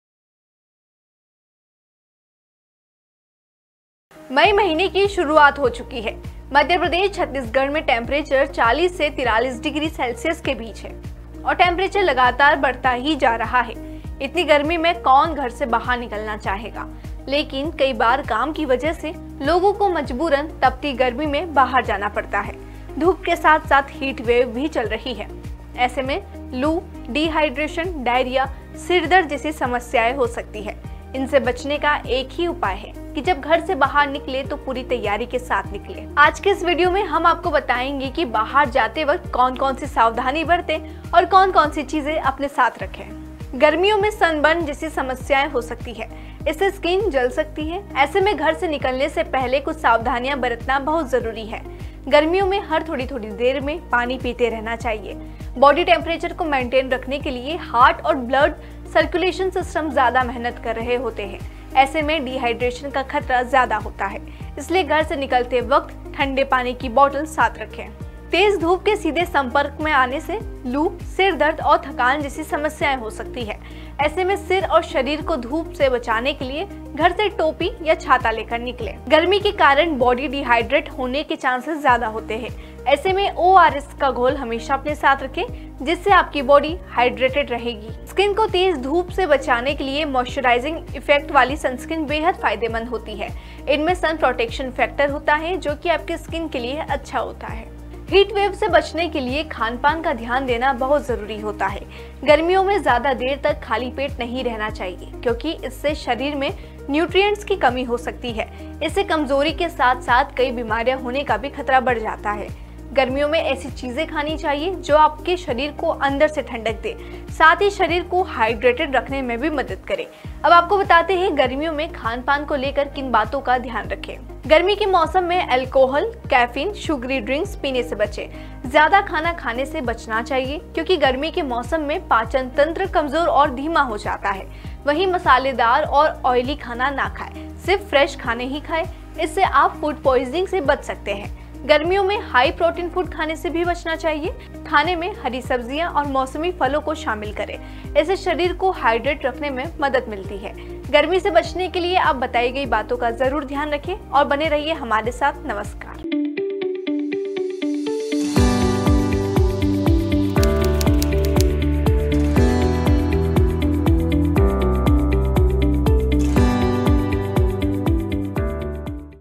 Summer Health Tips : इस चिलचिलाती गर्मी में घर से निकलने से पहले करें ये काम ।
गर्मी का मौसम शुरू हो चुका है। ऐसे में तेज धूप और लू से बचना काफी जरूरी है। आज के इस वीडियो में हम आपको बताएंगे कि आप कैसे तेज धूप और लू से खुद का बचाव कर सकते हैं ताकि आप बीमार न पड़े। गर्मियों में डिहाइड्रेशन की समस्या आम बात है, इसलिए पानी जरूर पीते रहें।

वीडियो अंत तक देखें | धन्यवाद |

The Sootr
Atarangi Media house private limited
3, Indira Press Complex,